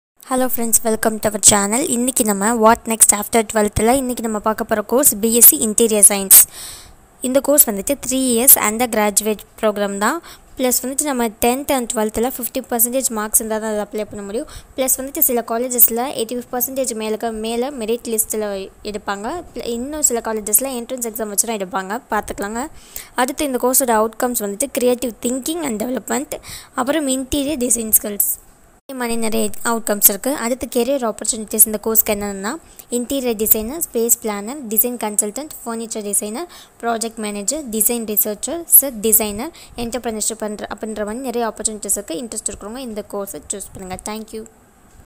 Career Opportunities for B.Sc. Interior Design | Career Option
Hellow friends!!!

This video is discussed about the topic

What are all the courses that we can choose after completing the  standard 12th ??

The course discussed in this video are

B.Sc. Interior Design

Full details about this course and what are the scopes after completing this course is discussed.

Thanks for choosing my video to watch my dear friends!!